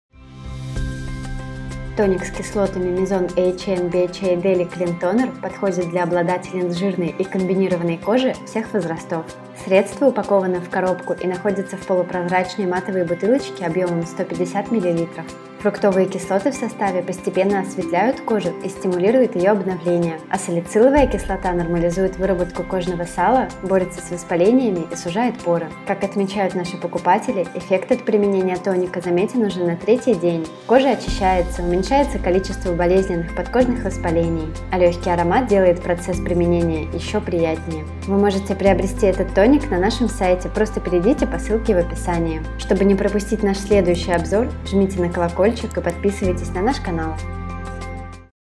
Обзор на тонер Mizon AHA And BHA Daily Clean Toner
Тоник с отшелушивающими AHA и BHA-кислотами Mizon AHA And BHA Daily Clean Toner подходит для обладательниц жирной и комбинированной кожи всех возрастов.
Фруктовые кислоты в составе постепенно осветляют кожу, растворяют отмерсшие клетки кожи , стимулируют ее обновление. А салициловая кислота нормализует выработку кожного сала, борется с воспалениями и постепенно сужает поры.

Купить тонер Mizon AHA And BHA Daily Clean Toner по супер-цене вы можете на сайте LuckyCosmetics.ru
Тут вас ждет много интересных видео! До новых встреч! ;)